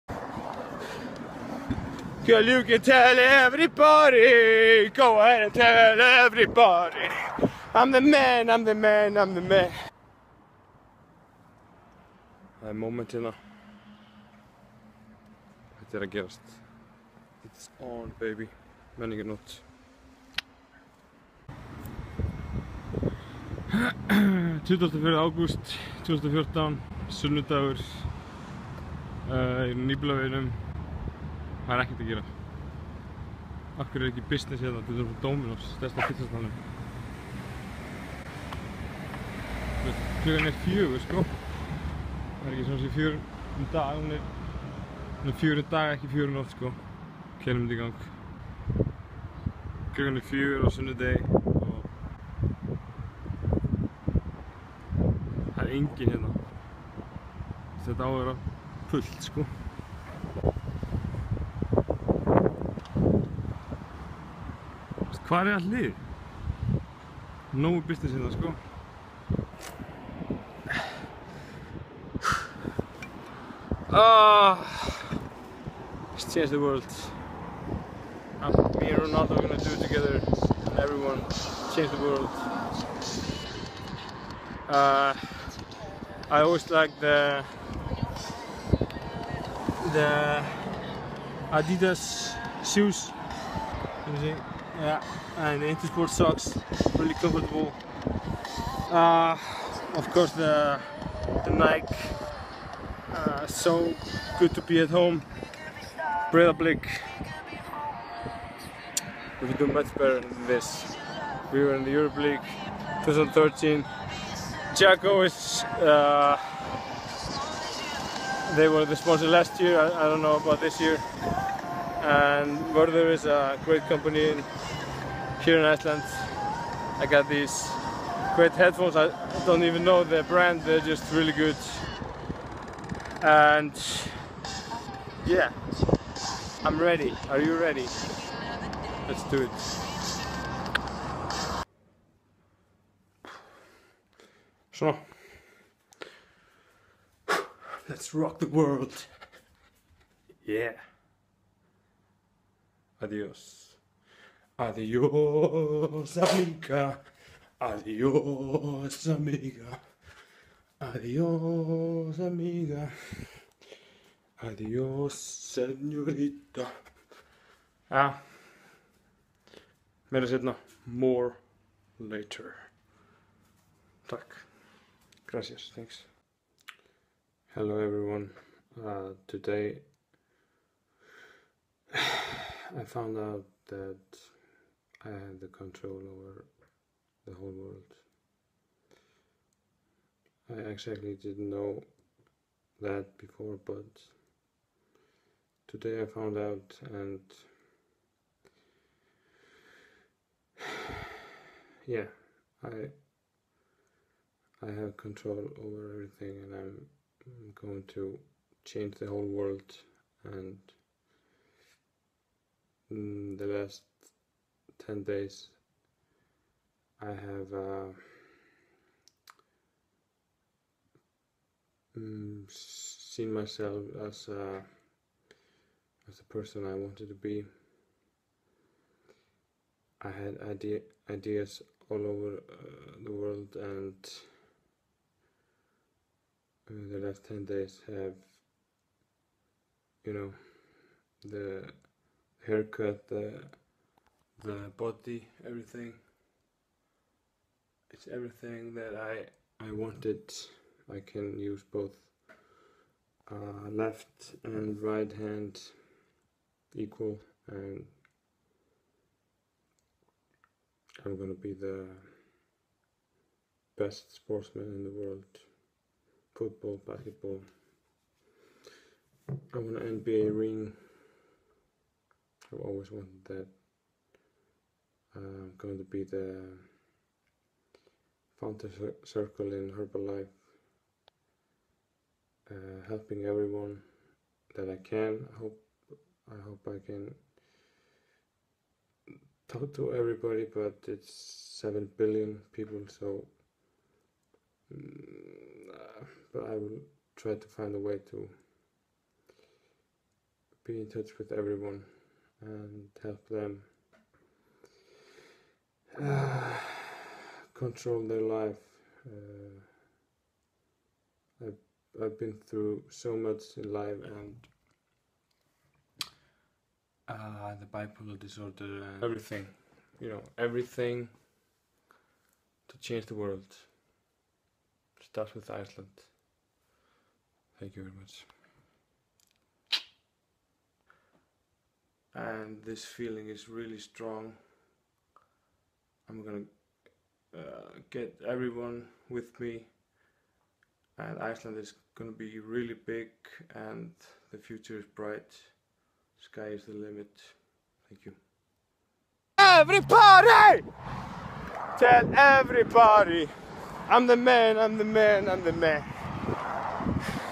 The Man who was going to conquer the World (Manic episode)
Manic episode in August 2014. Most of the time about changing the world, but it only lasted 12 days that time. Manic episode can be a lot of fun but it can be dangerous at the same time. If people are manic for a long time without treatment they could think they´re unstoppable and maybe jump down from high building cause nothing can beat them. The reason why I´m posting this video is for people to see how this situation can be. Around 90% people (in Iceland) have no idea what mania is but everyone knows something about the other side, the depression